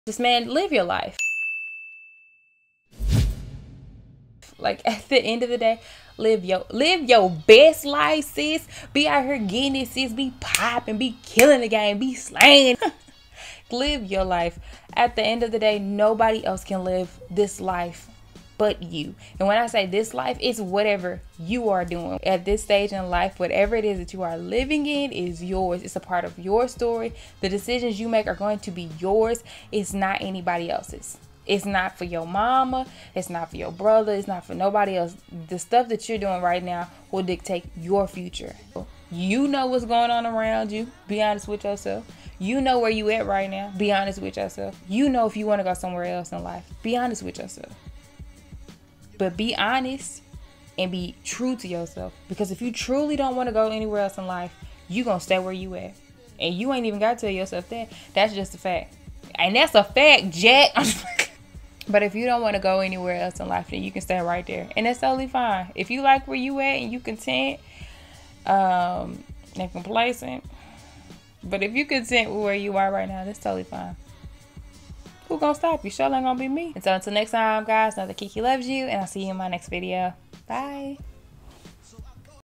Be honest with yoself!
The best way to be is honest! Often times lie to ourselves about what we want to do and where we want to be. We say we want the best and we do... but we don't really want it well. what it requires.

Let this be a reminder to go get it!